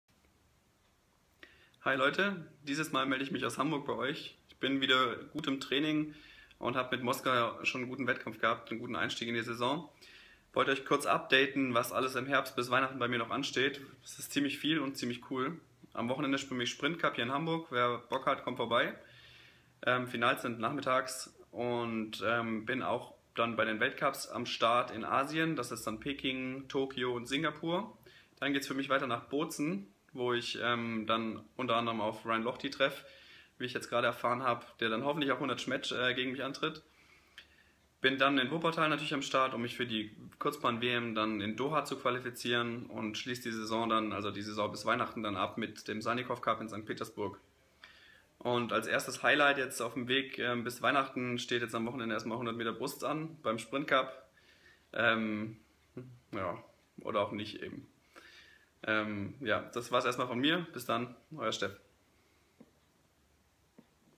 Update Wettkampfkalender
Was steht alles noch auf meiner Wettkampfagenda bis Weihnachten? Ziemlich viel:
Sprintcup Hamburg
Weltcuptour Asien (Peking, Tokio, Singapur)
Schwimmfest Bozen
DM Wuppertal
WM Doha
Salnikov-Cup St. Petersburg